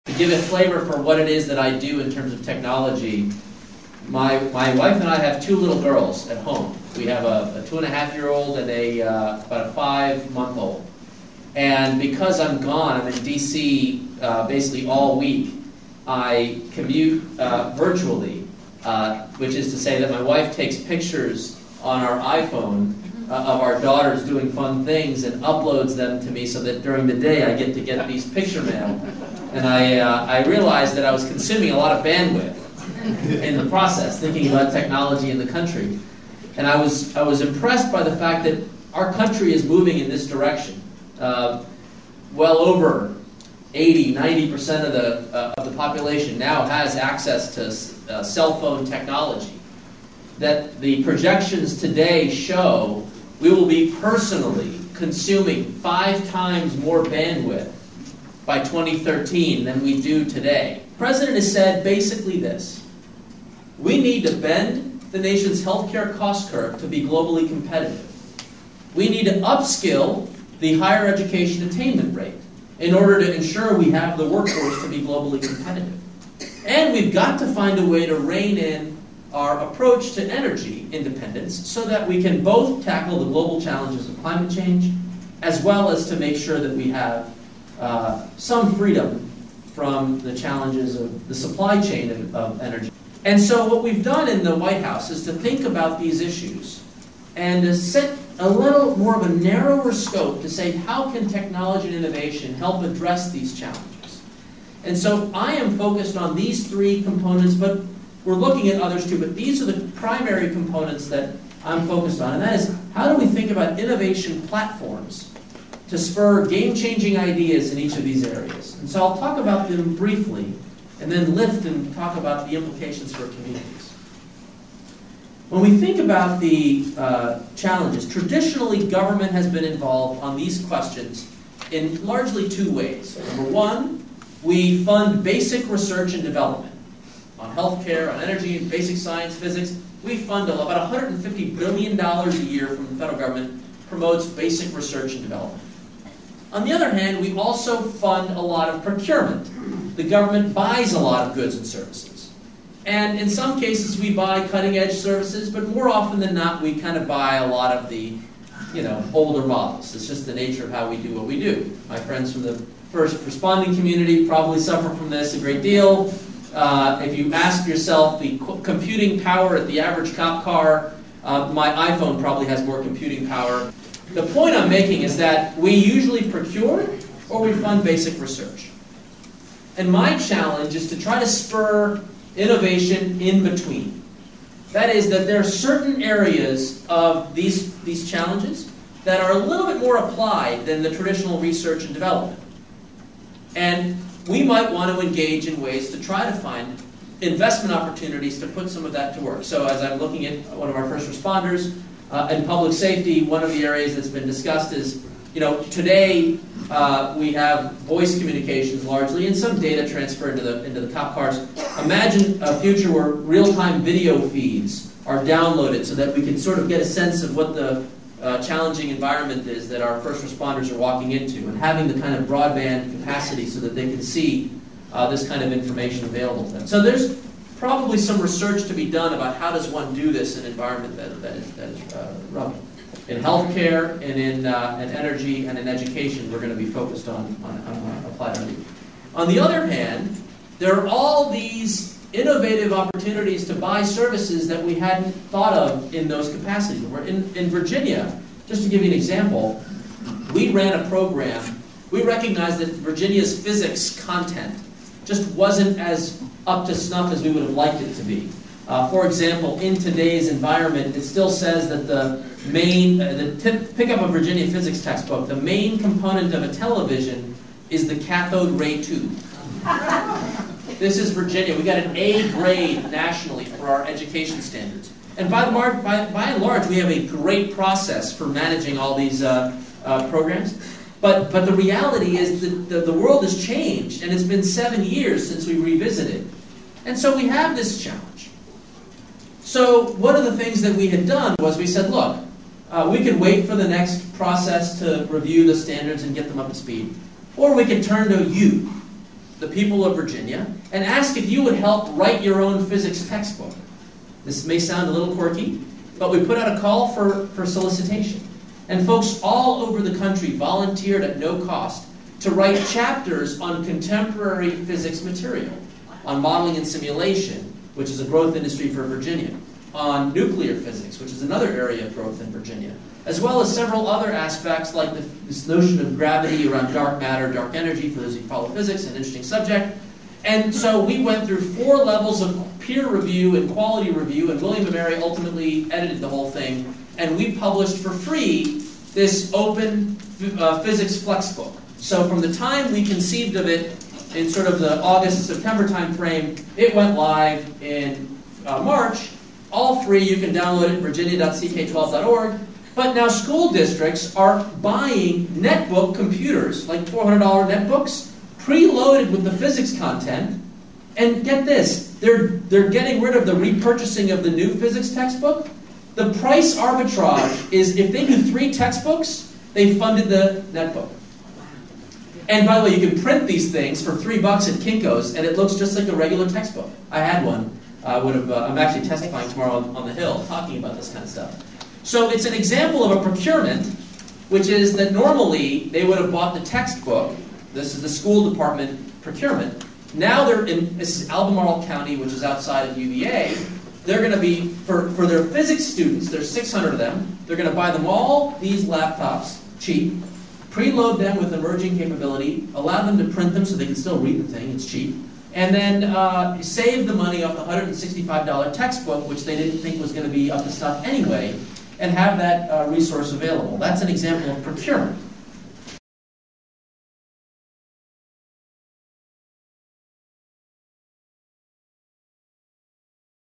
Aneesh Chopra, U.S. Chief Technology Officer discusses priorities - Part 1
Aneesh Chopra discusses what he will do as President Obama's Chief Technology Officer.  His focuse will be on three areas -- health care, education, and enviromental issues.  Part I includes his discussion of Virginia's innovative participatory development of a new physics textbook.